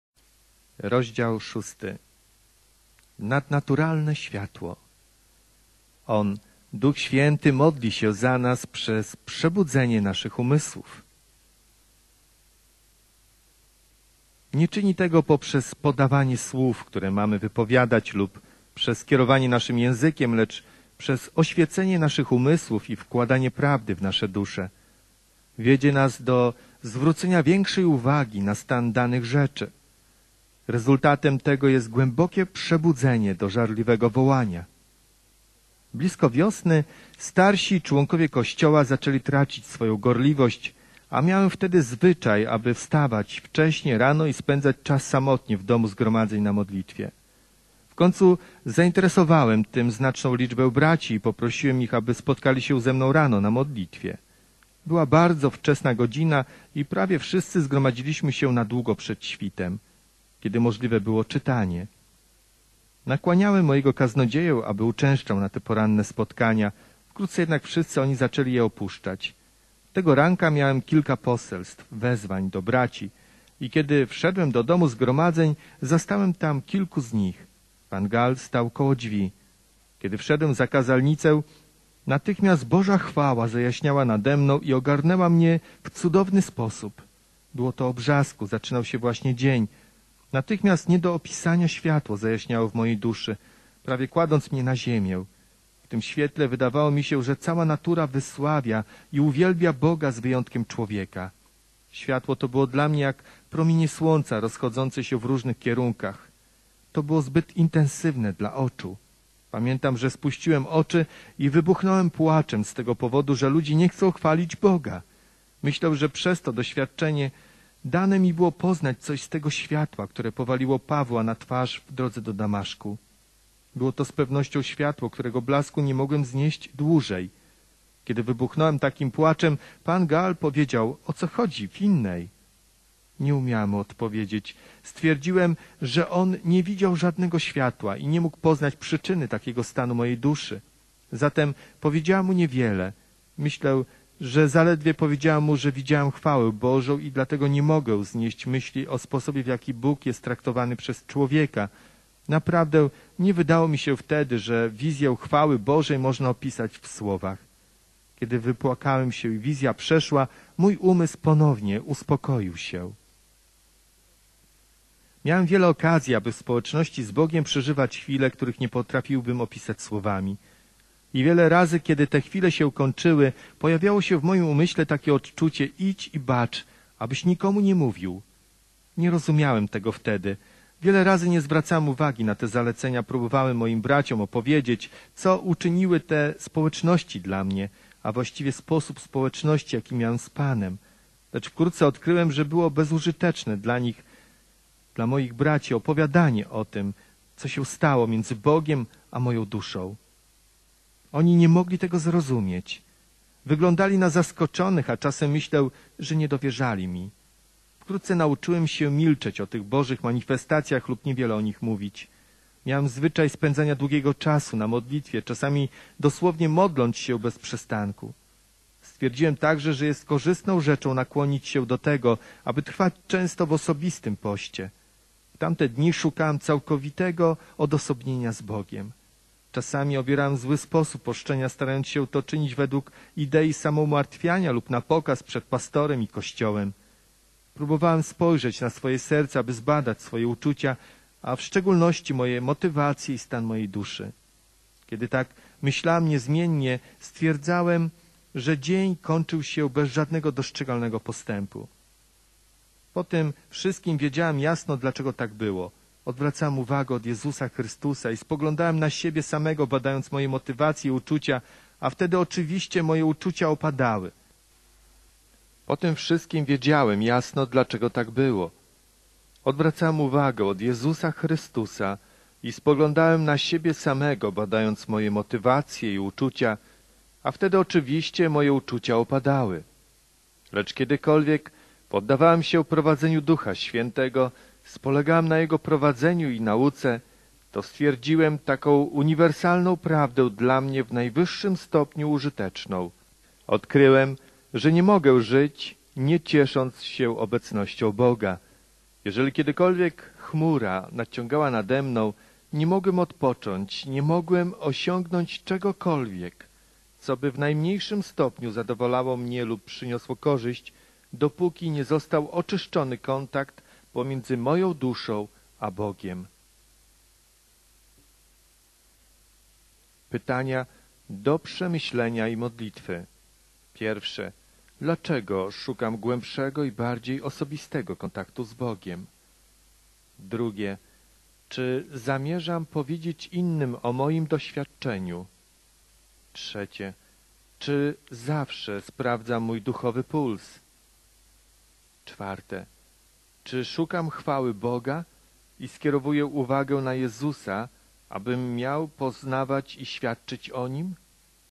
06 C.G.Finney - Odpowiedzi na modlitwę - Nadnaturalne światło.
Przezwyciężająca i skuteczna modlitwa, to taka modlitwa, która osiąga błogosławieństwo, którego szuka. Jest to taka modlitwa, która skutecznie porusza Boga. Prawdziwym celem skutecznej modlitwy jest to, aby poruszyć swój obiekt. ("Z zasad modlitwy"  Ch.G. Finney'a)